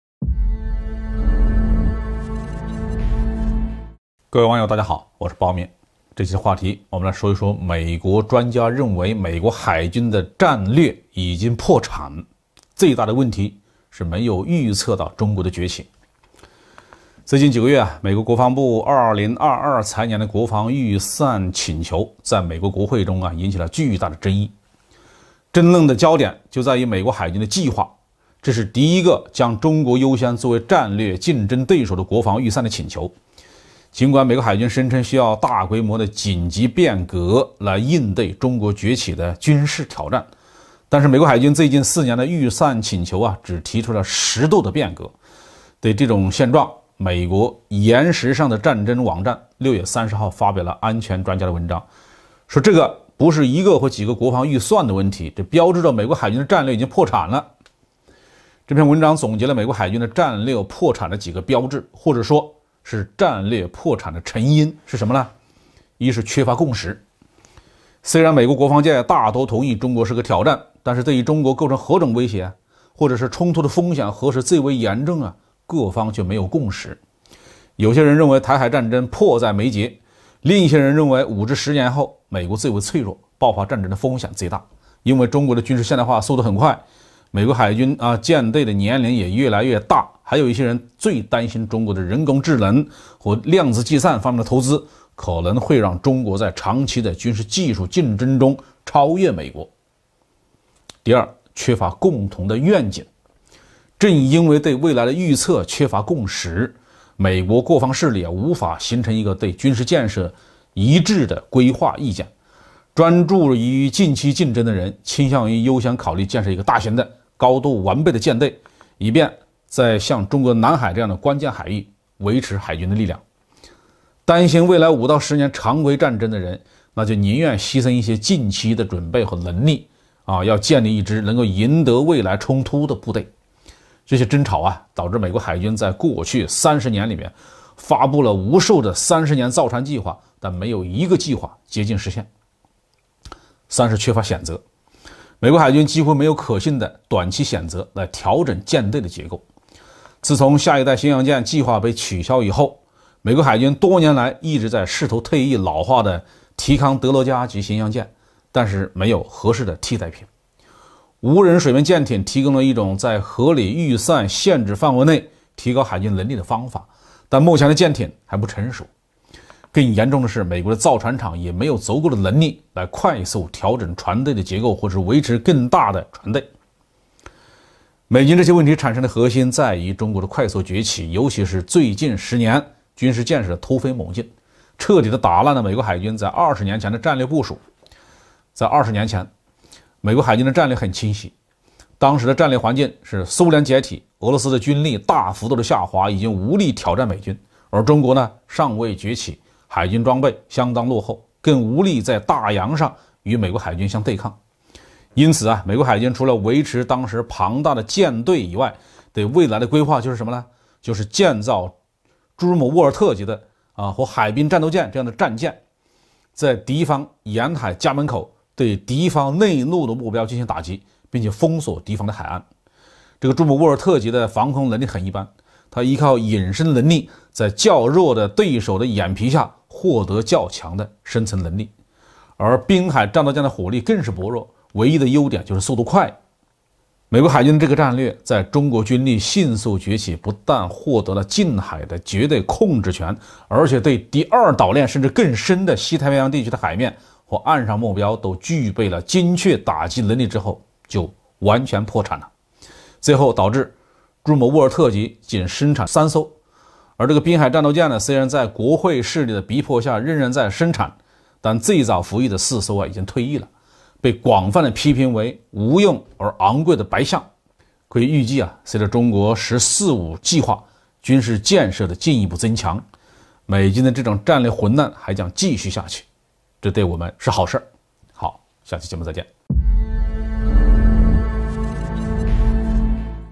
美國專家：美國海軍的戰略已經破產，萬萬沒有預測到中國的崛起​​！【包明】
美國專家認為美國海軍的戰略已經破產，最大的問題是沒有預測到中國的崛起
美國專家：美國海軍的戰略已經破產，萬萬沒有預測到中國的崛起​​！
政治 軍事 包明 在我們的影片中，我們會特別關注避免任何可能引起仇恨或衝突的言論或情節。